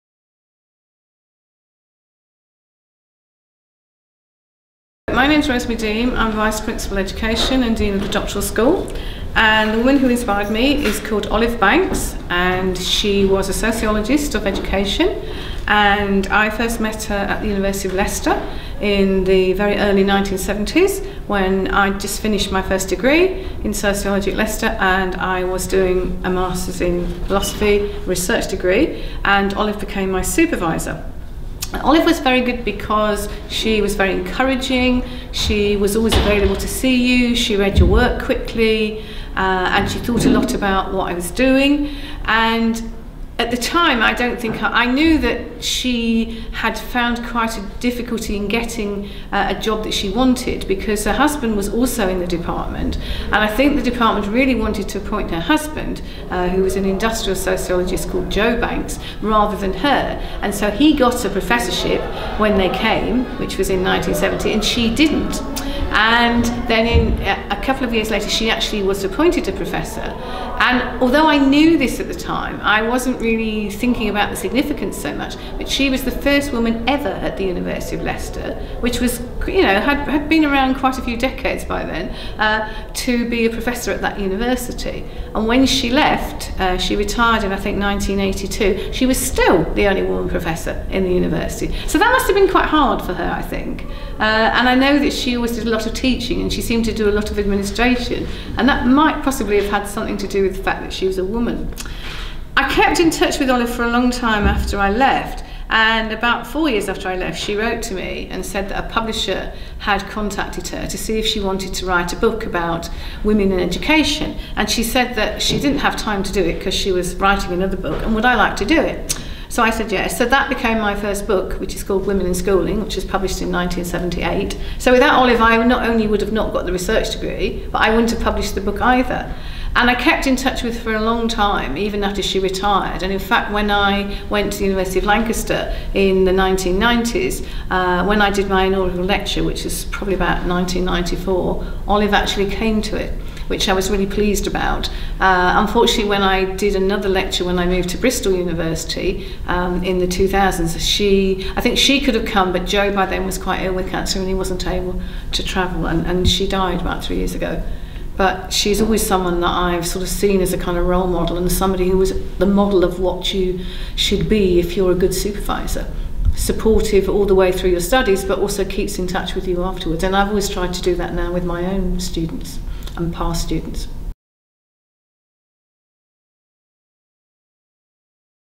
Women Inspire: Rosemary Deem
Professor Rosemary Deem, Vice Principal (Education) and Dean of Doctoral School, tells us about the woman who inspires her most, Olive Banks. Olive Banks (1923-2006) was the first female professor at the University of Leicester and on her retirement in 1982 she remained the only female professor at Leicester.